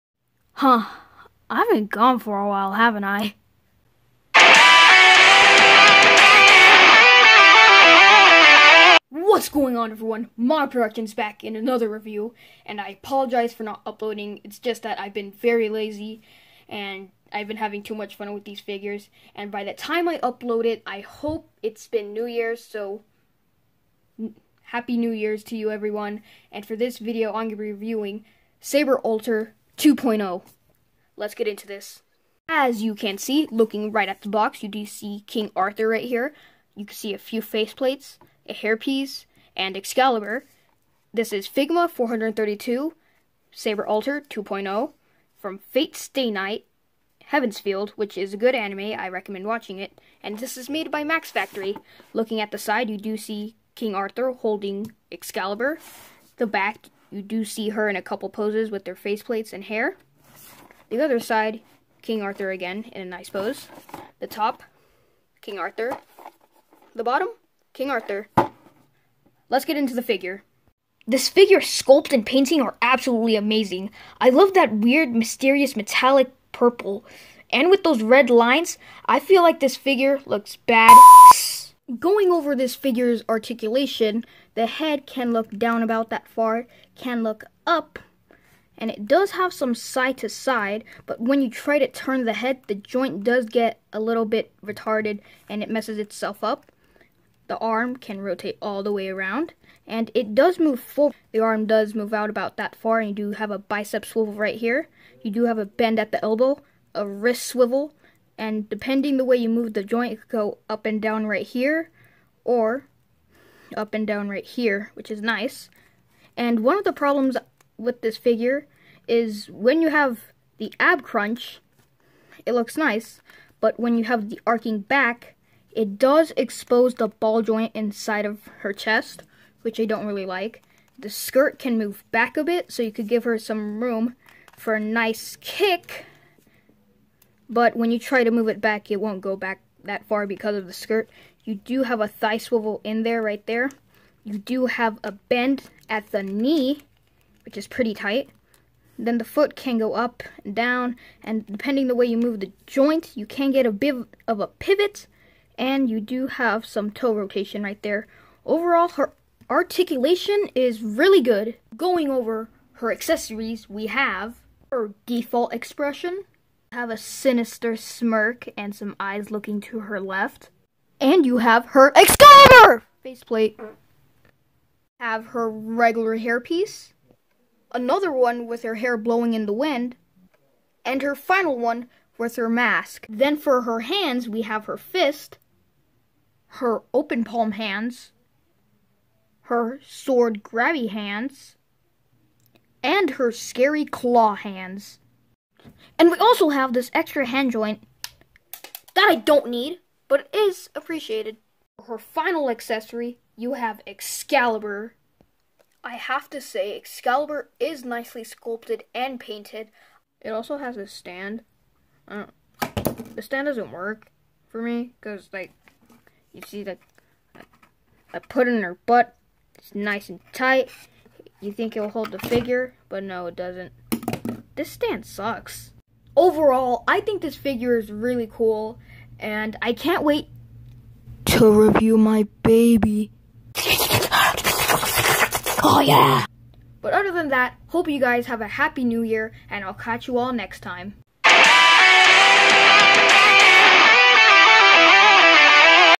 Figma Saber Alter 2.0 Review
What's going on if you want Mana Productions back in another video. In this video I'm gonna be doing a review on the Figma Saber Alter.

NEXT REVIEW:

ANIME HEROES NARUTO UZAMAKI FIGURE

KNOCK OFF SH FIGUARTS MINATO

Figma Alter Jeanne d'arc (Jalter)

If you have any figure recommendations leave them in the comments below!